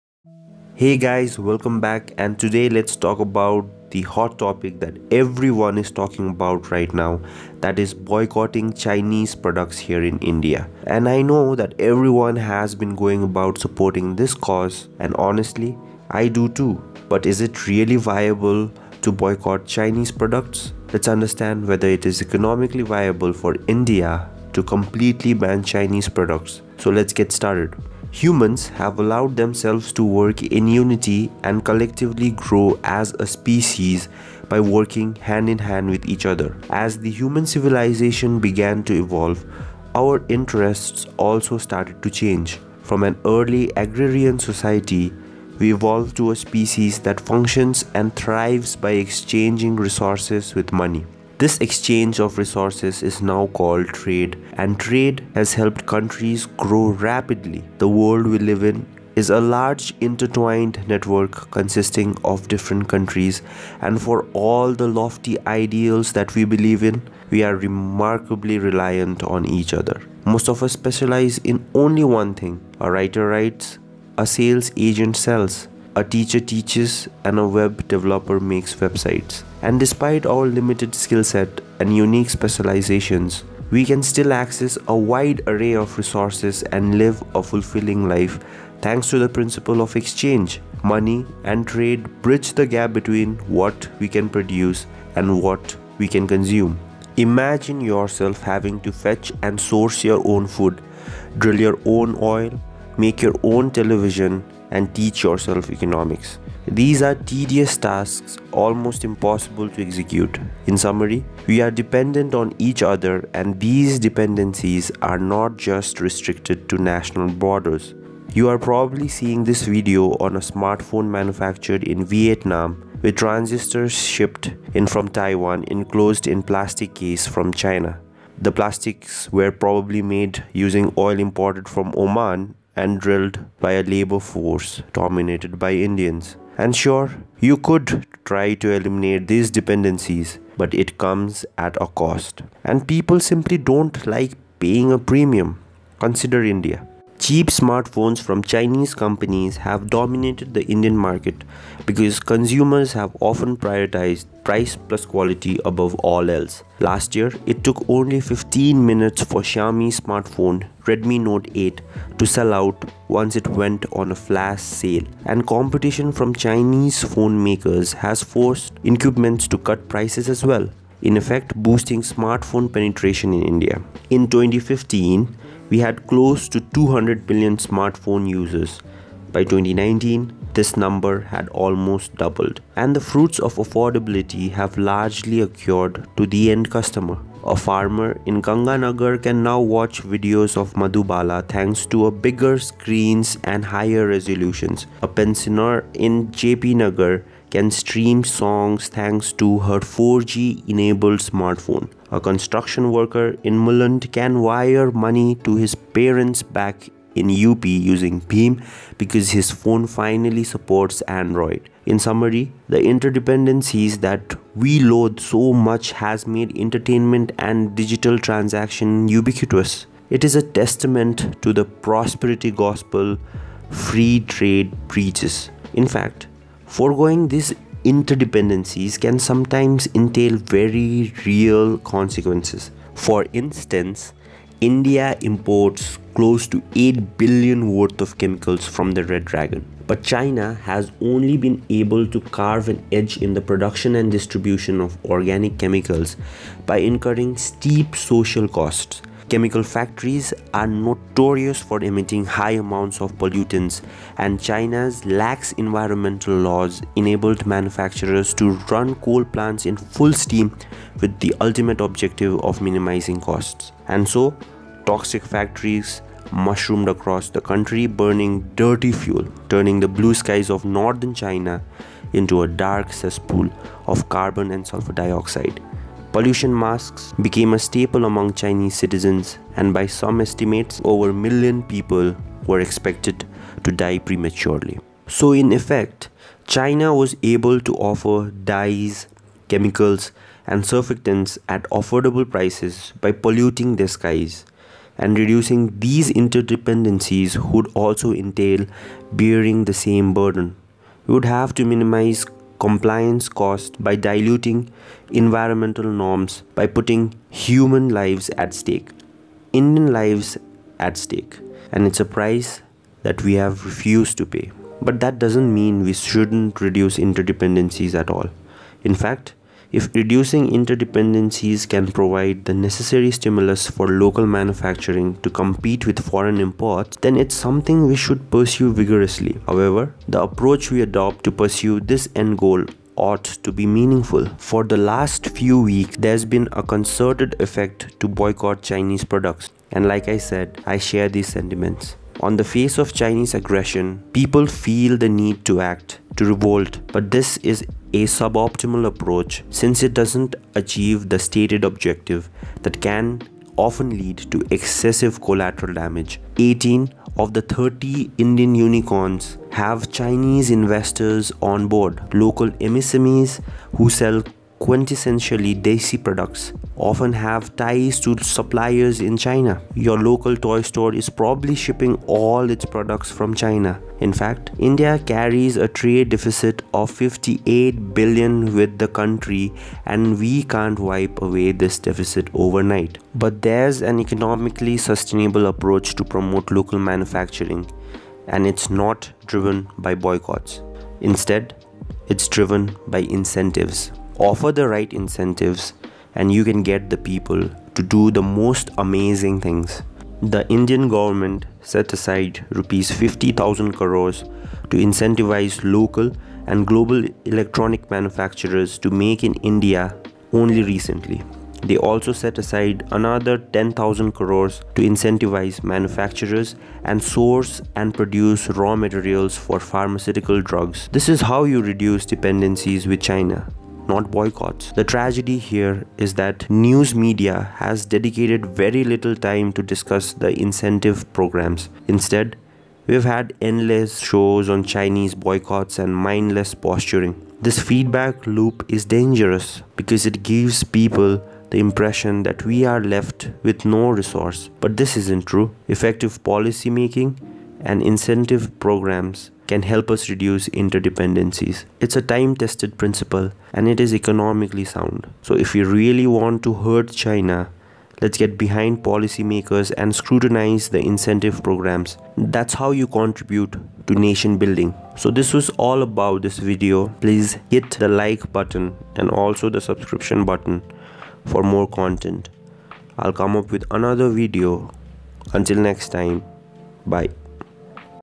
"BOYCOTT MADE IN CHINA ?" || Here's the right way
This video talks about  the ongoing issue between India and China and what kind of economic toll it can take if we completely ban Chinese products.

A breakdown on how the country should tackle this issue is briefly discussed.

To get a complete idea , I urge you guys to finish watching this video and give your views on the comment sector.